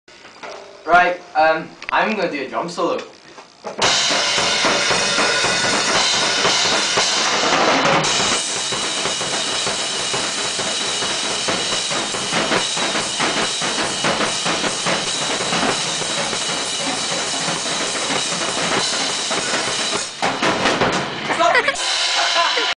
Drum Solo 1
This is our drummer's 1st drum solo. It's amazing...until he drops his drum stick.